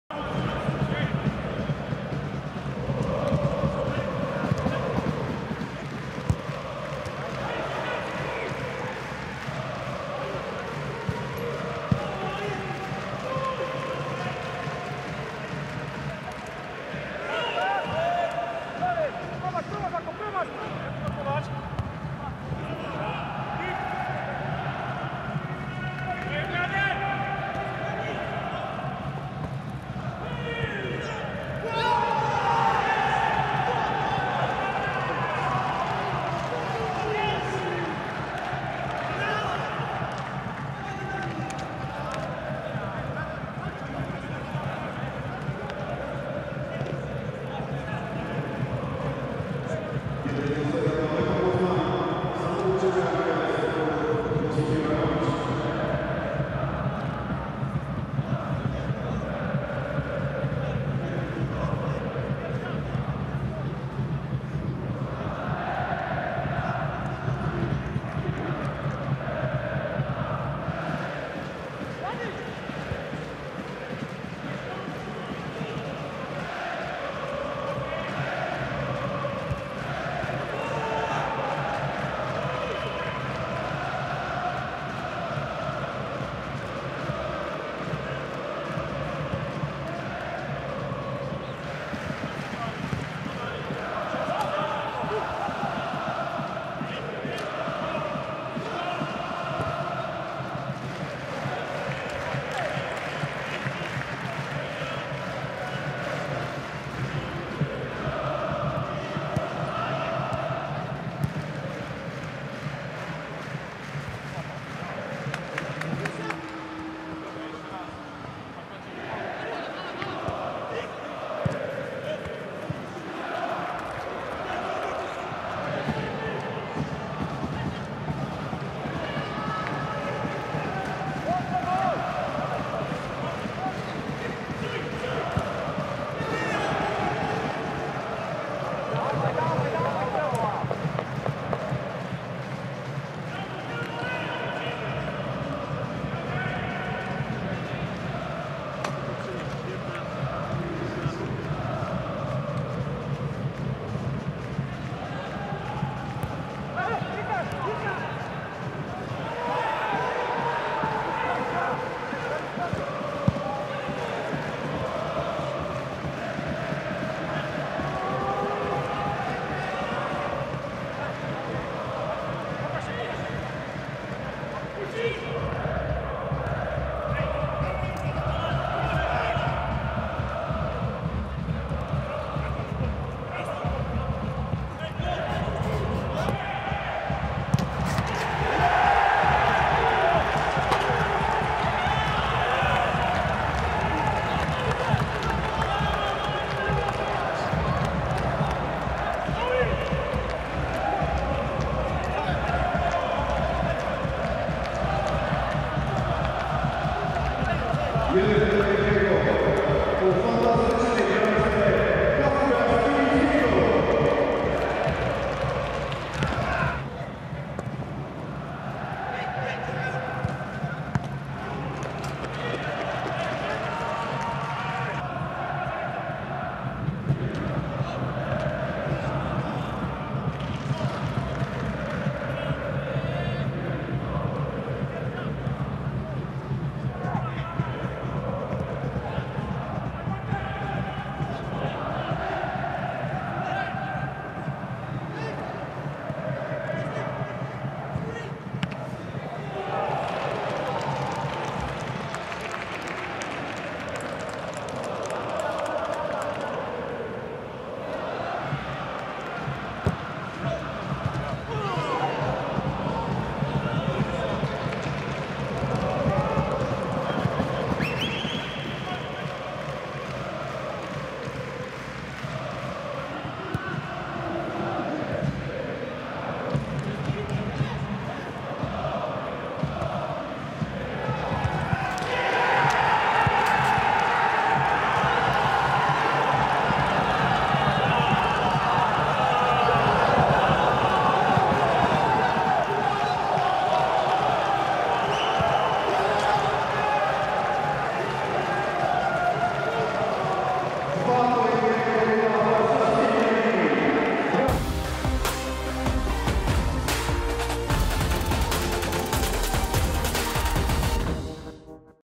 Legia Warszawa - Lech Poznań 2:1 | SKRÓT | Ekstraklasa 2020/21 | 9. Kolejka
Legia Warszawa 2:1 Lech Poznań
0:1 - Josip Juranović (29') (s)
1:1 - Kacper Skibicki (67')
2:1 - Rafael Lopes (90')

EKSTRAKLASA TV ► Mecze na żywo za granicą